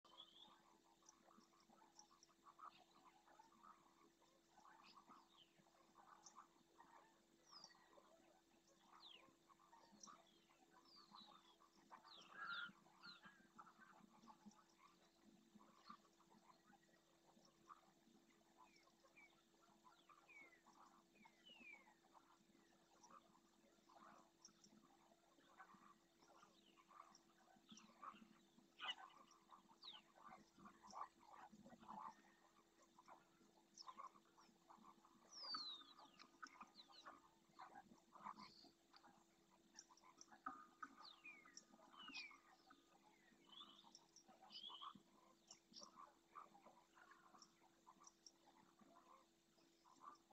Focha común ANILLADA - Parque Nacional de Doñana (marisma); España
Un bando de fochas comunes se está alimentando en el borde de la laguna. Una de ellas está anillada y lleva una marca de lectura a distancia, un collar negro con la inscripción en blanco 190.

Gracias al registro de la observación a través de la EBD (Estación biológica de Doñana) ha sido posible conocer su historial.

Nació en la Cañada de los Pájaros (Sevilla), fue anillada el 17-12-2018 en su primer año de vida (Edad Euring 3)

La próxima primavera la focha cumplirá cinco años.

Es la primera vez que se ha registrado una observación de esta focha, desde su anillamiento.

¿Quieres saber más sobre el anillamiento científico de aves? Toda la información en el siguiente enlace